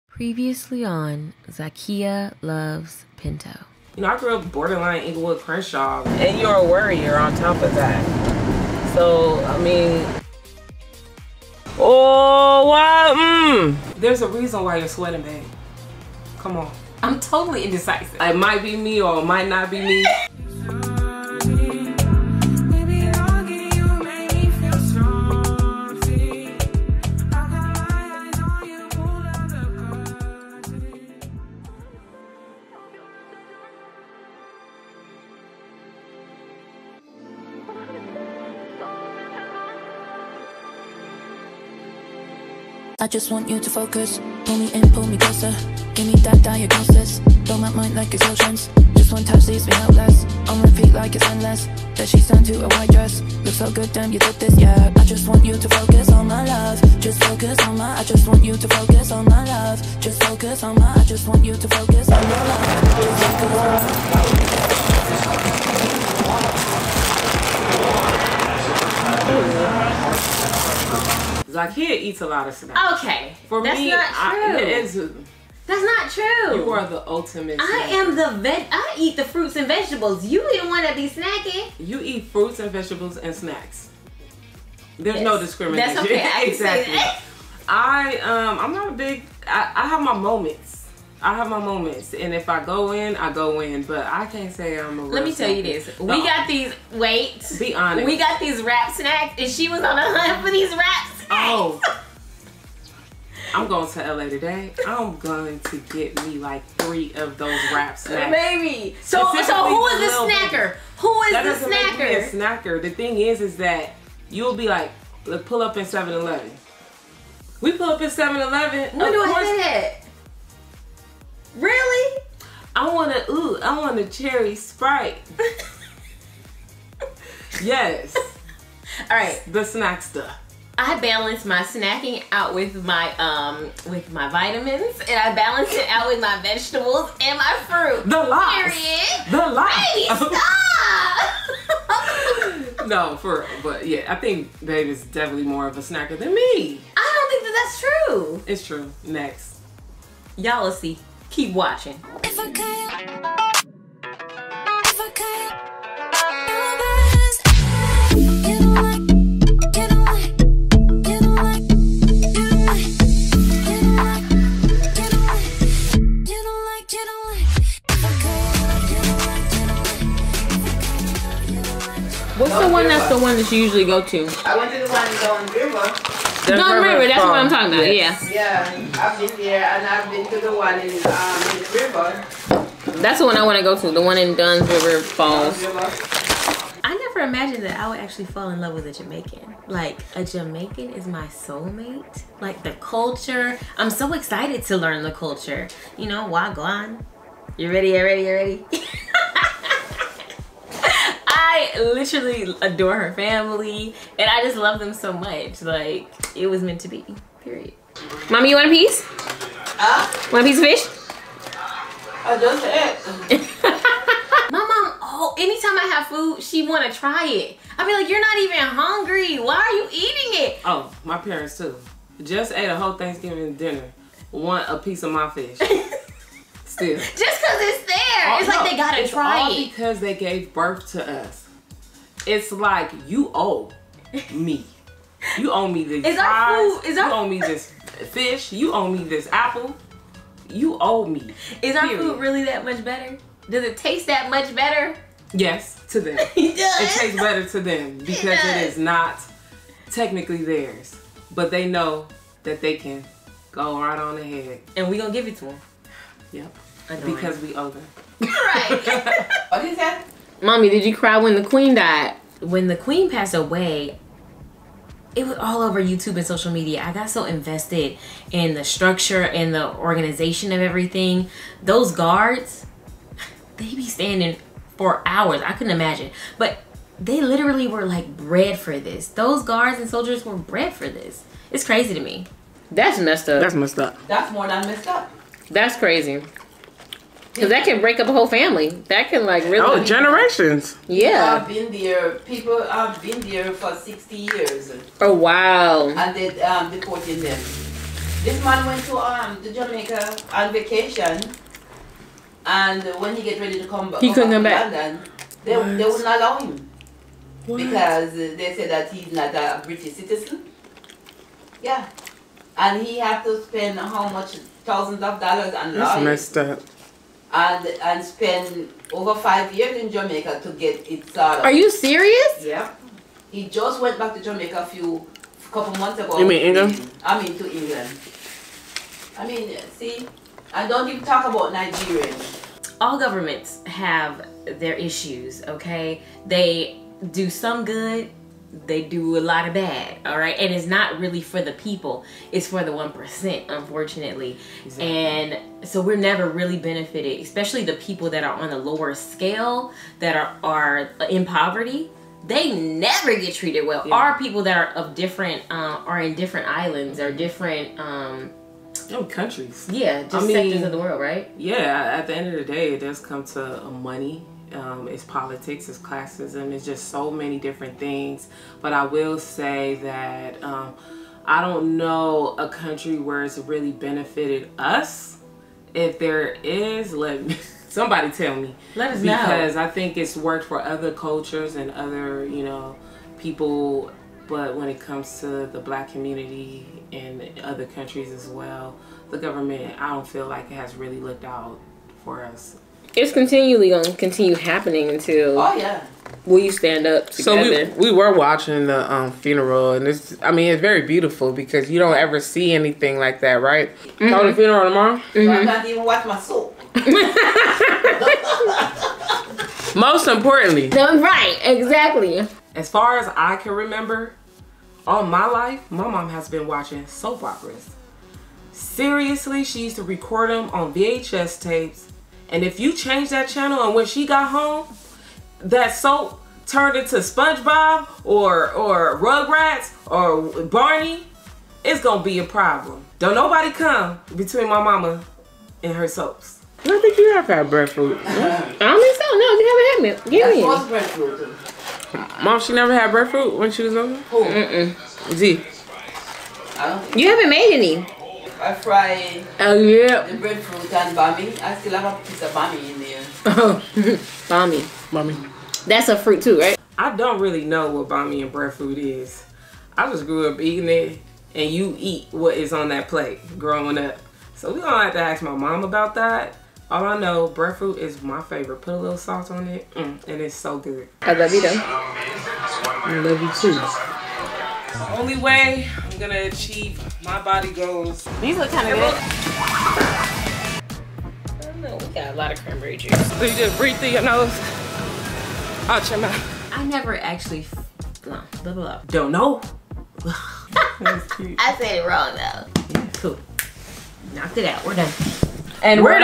VLOGTOBER : LUNCH with the QUEEN of ENGLAND Episode 3
Finally. It's VLOGTOBER! and we are bringing you new content everyday! We hope you guys are entertained because this is only the beginning.

Episode 3 ~ Zakiya and Pinto grab food at Happy Fish. They head over to the Pinto Family home in Crenshaw to spend time with Mom and the discussions get heated.

RECENT TRAVEL VLOGS ⇣

Love Y'all
ZakiyaLovesPinto
Keep up with ZakiyaLovesPinto:
INSTAGRAM: @ZAKIYALOVESPINTO | @IAMZAKIYAASHA | @NXPINTO